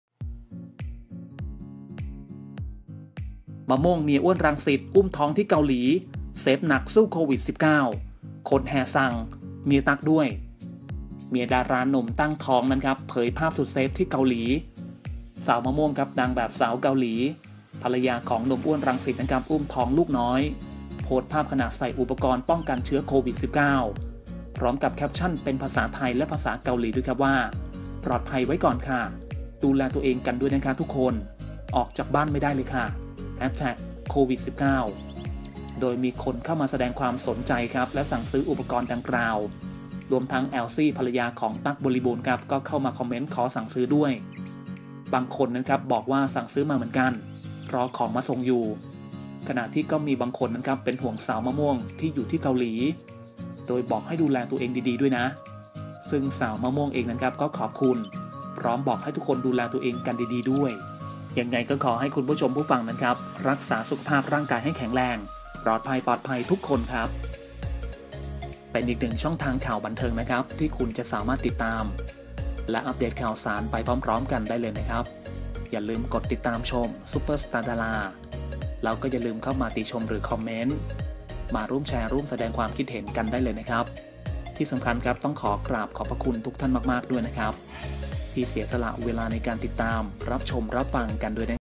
มะม่วง เมีย อ้วน รังสิต อุ้มท้องที่เกาหลี เซฟหนัก สู้โควิด19 คนแห่สั่ง เมียตั๊กด้วย#super star dara
มะม่วง เมีย อ้วน รังสิต อุ้มท้องที่เกาหลี เซฟหนัก สู้โควิด19 คนแห่สั่ง เมียตั๊กด้วย
เมียดาราหนุ่มตั้งท้องเผยภาพสุดเซฟที่เกาหลี - มะม่วง นางแบบสาวเกาหลี ภรรยา อ้วน รังสิต อุ้มท้องลูกน้อย โพสต์ภาพขณะใส่อุปกรณ์ป้องกันเ